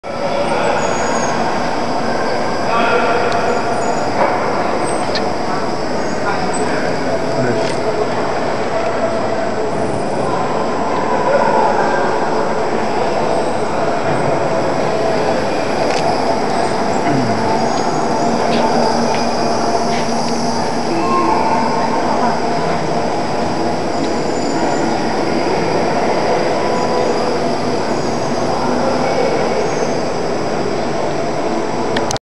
Paddle wheel london2012
Paddle Wheel in Maritime Museum, London (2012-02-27).